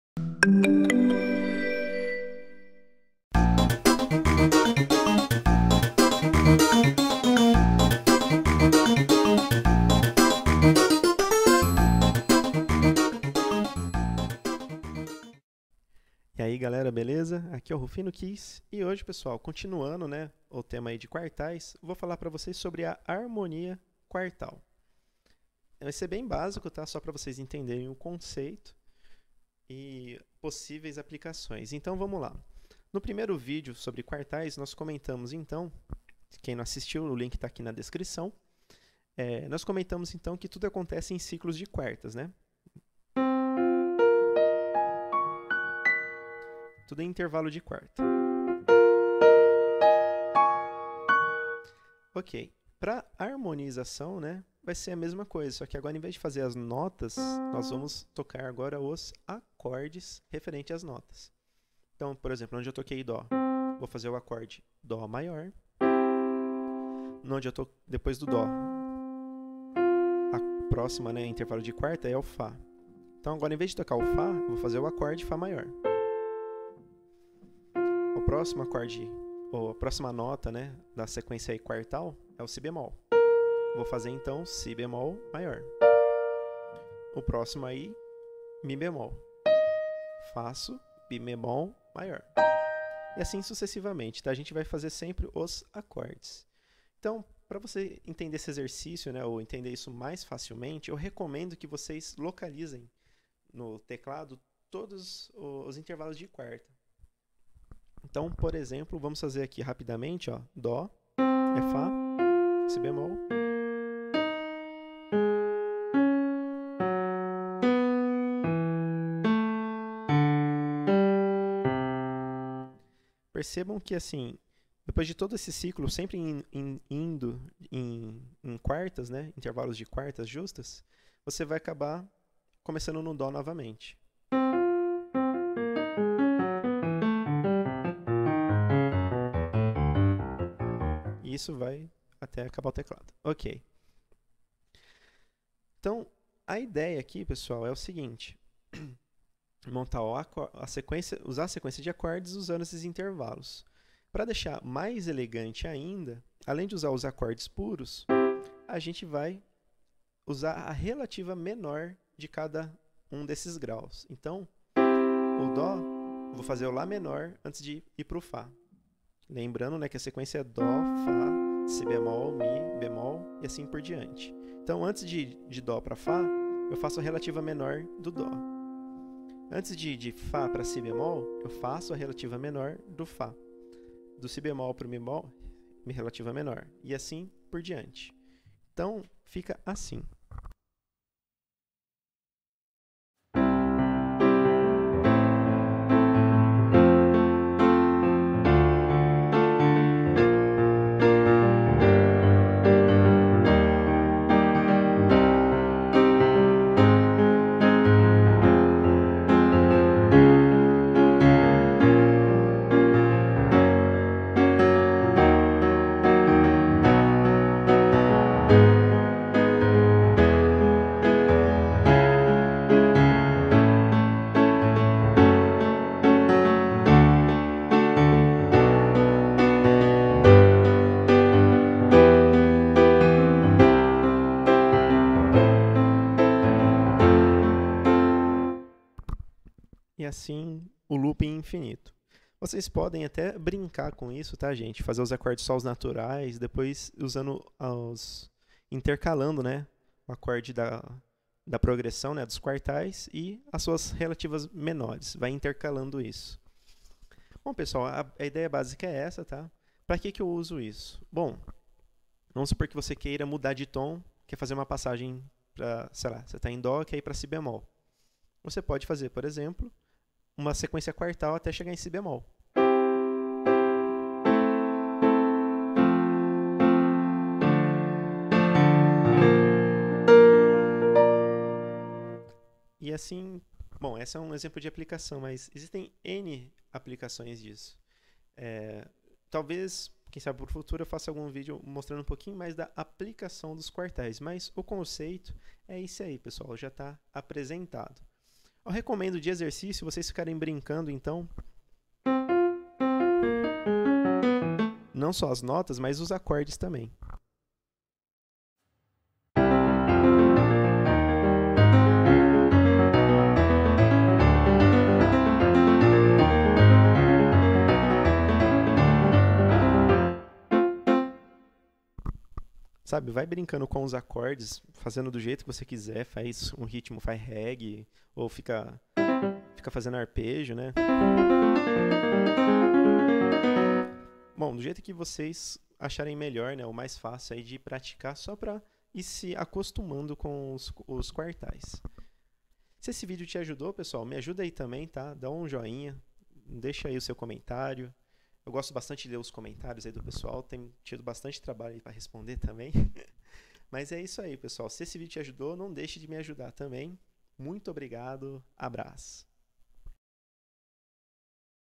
Teclado Mais Fácil - Progressão Harmônica - Quartal
Este é o primeiro vídeo sobre progressão harmônica do canal. A sequência de acordes utilizada na harmonia quartal segue a mesma "matemática" dos acordes quartais, com adição de um detalhe: para cada grau quartal toca-se seu respectivo acorde. Uma forma de evoluir um pouco mais a harmonia quartal é a adição de acordes relativos menores entre cada grau quartal - conforme o demonstrado no vídeo.

Espero que este vídeo tenha ajudado a compreender um pouco mais sobre o assunto. ^.^

Abrasssss o/

Rufino Keys